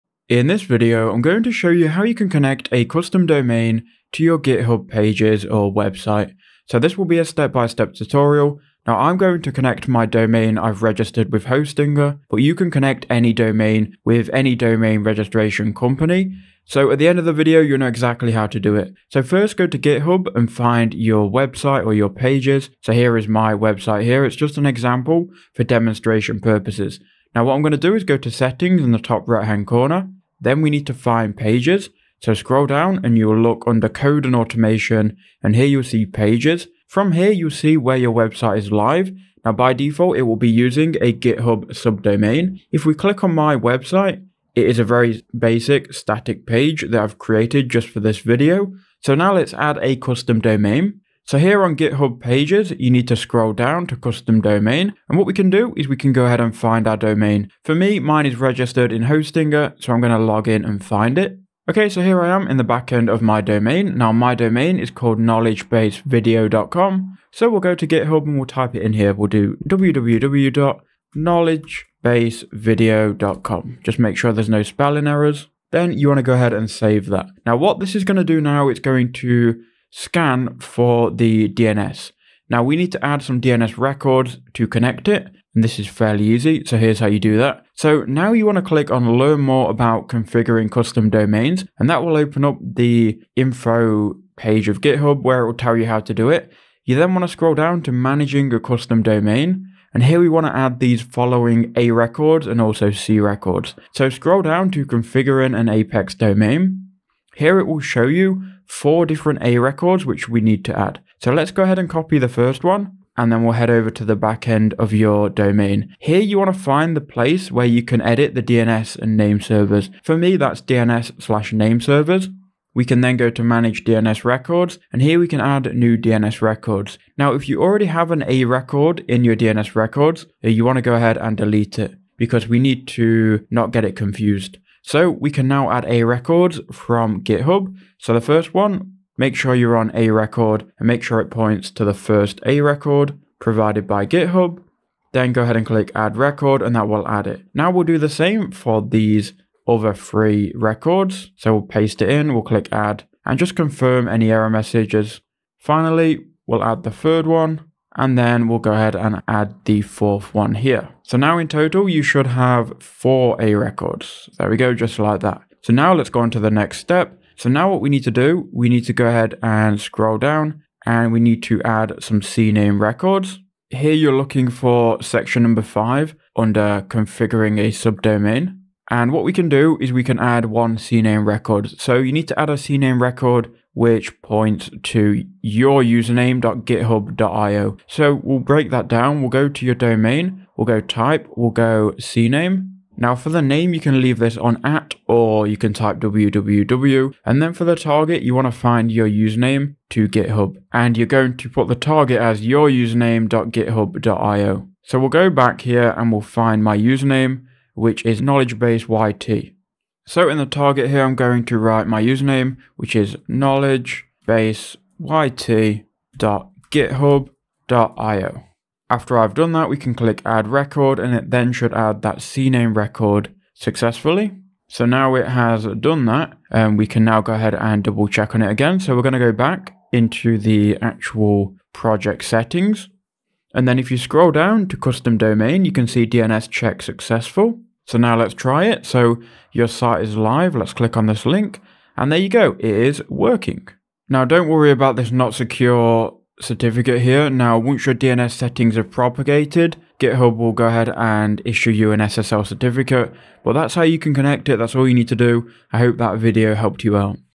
How To Connect Custom Domain To GitHub Pages (Step By Step)
In this video, I show you how to connect custom domain to GitHub pages. Did this video help? Thanks!